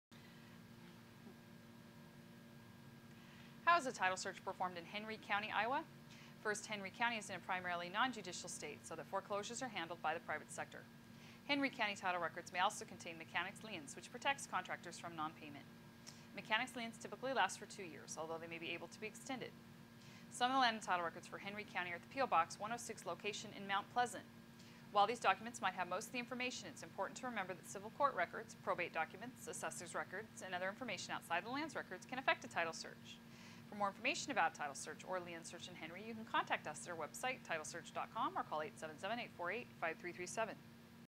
Property title records in Henry County Iowa | AFX
Here is some information on property title records in Henry County Iowa. In this video we will discuss performing a title search in Henry County Iowa.